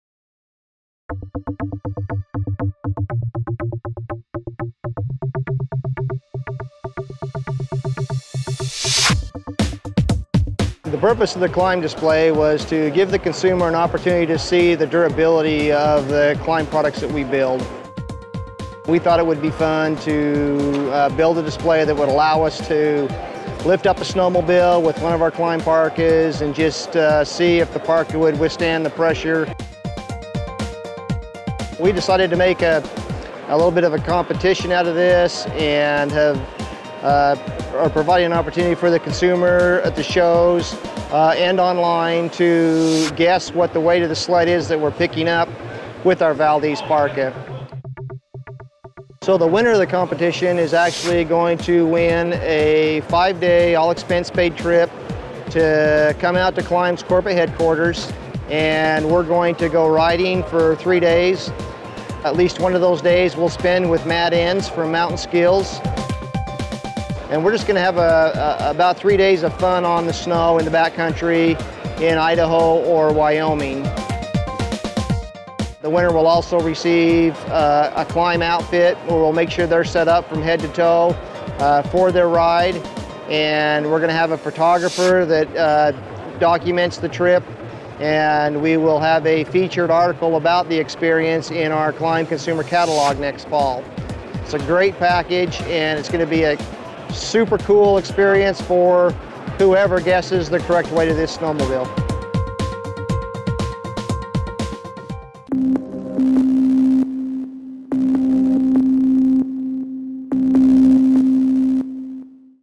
Snowmobile hangs from KLIM jacket
Now that's durability! Performance, withstanding wear and tear... yeah, we know it and we are now showing it off at various shows across the country!

Come check it out, and guess how much that sled weighs for a chance to win a snowmobiling trip of a lifetime! You can also guess online, here: